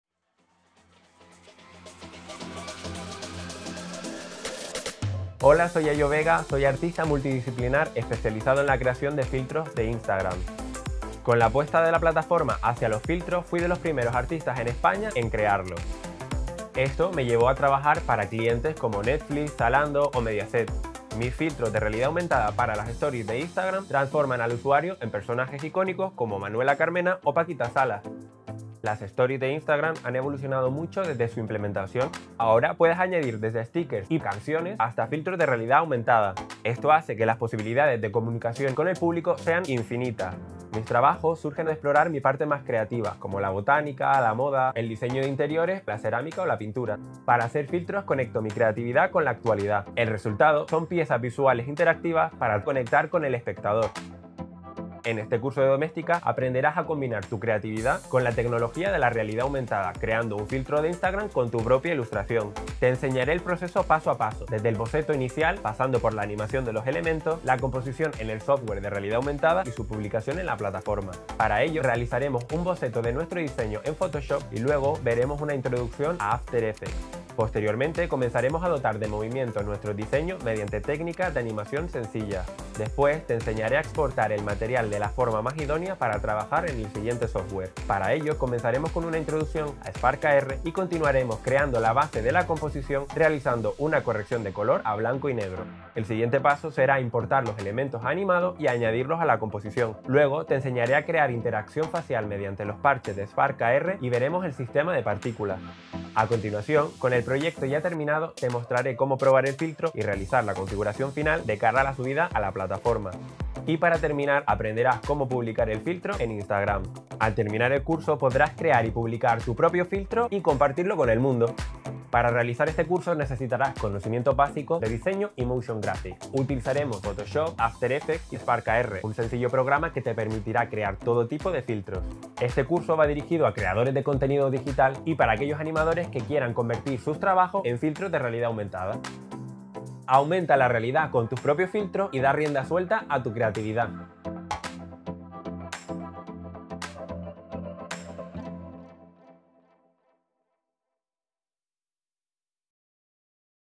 Filtros Animados en 2D para Instagram | Un curso de Ayo Vega
Aprende técnicas de animación para realidad aumentada con After Effects y Spark AR.

Los filtros de realidad aumentada 2D son un gran aliado para impactar a usuarios en Instagram como muestra Ayo Vega, artista especializado en la creación de contenidos digitales que ha creado una infinidad de filtros logrando conectar con una audiencia cada vez más amplia.

En este curso Ayo te enseñará a crear un filtro animado en 2D para Instagram pasando por todas las etapas del proceso creativo, desde el diseño hasta la composición animada en Adobe After Effects. Aprenderás a utilizar las principales herramientas del software Spark AR y pondrás en práctica los elementos de tu filtro para conseguir resultados óptimos y ofrecer una experiencia de usuario muy positiva y entretenida.